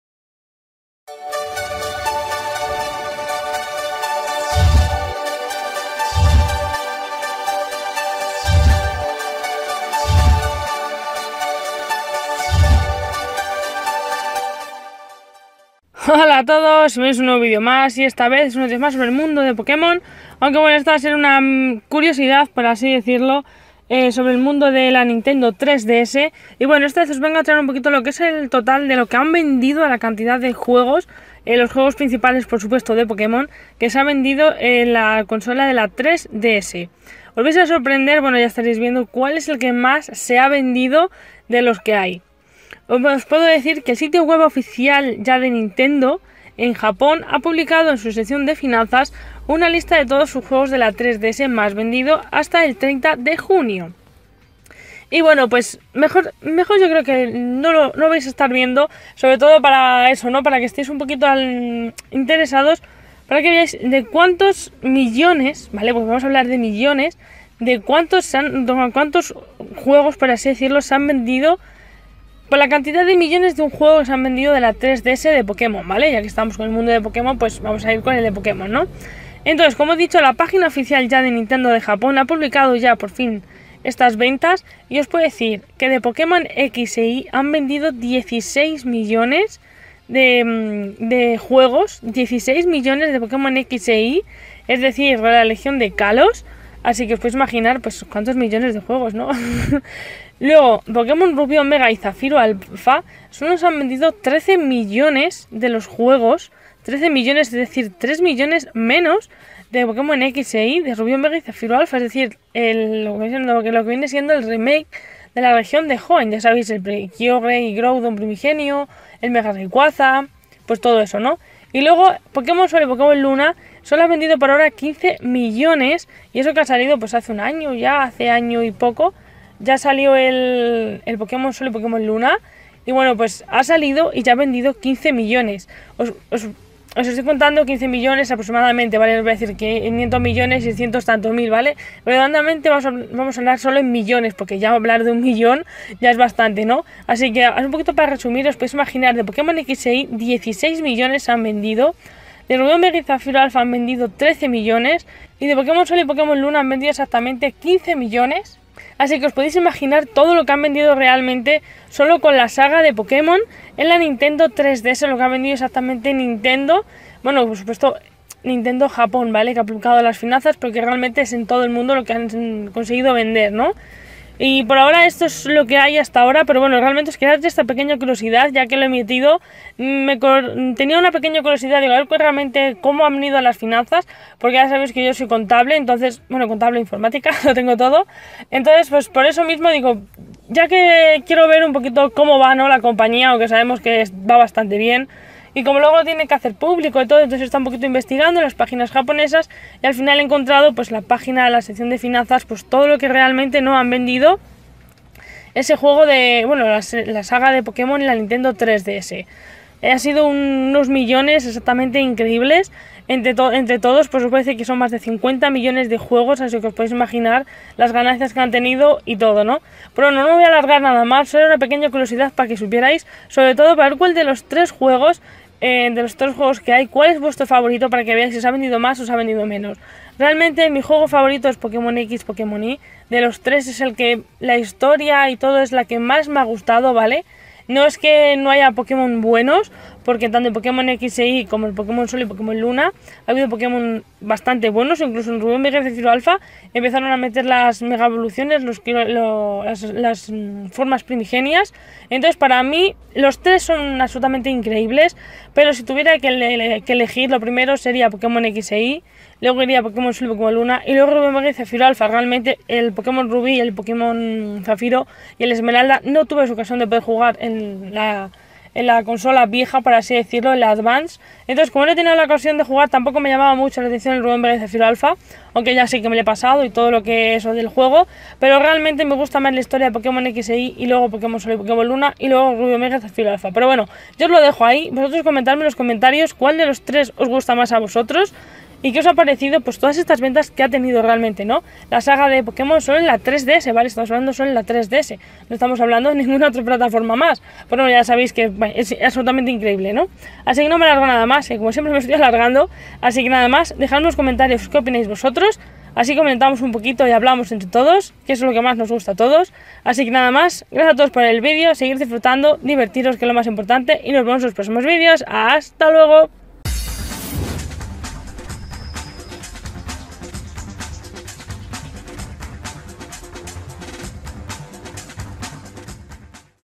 Cual es el mas vendido de los juegos principales de Pokémon para 3DS
Os cuento un poquito sobre cuál es el videojuego de Pokémon para la 3DS más vendido.